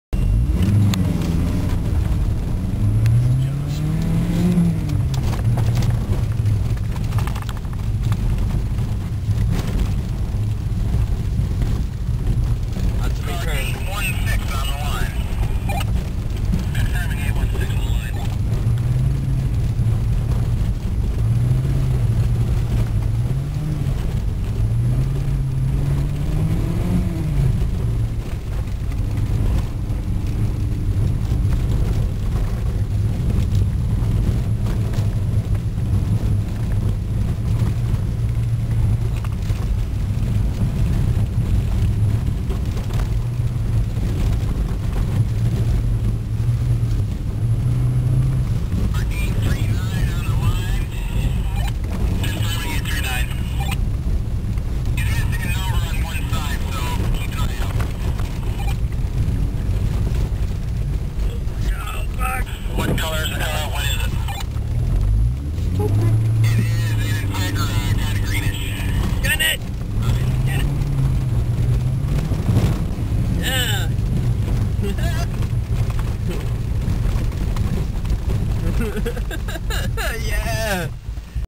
NER Rally 4th Run
My 4th run for NER Rally at New Hampshire Dragway, Epping, NH.  Running 89 VW GTI, 2.0L 16v.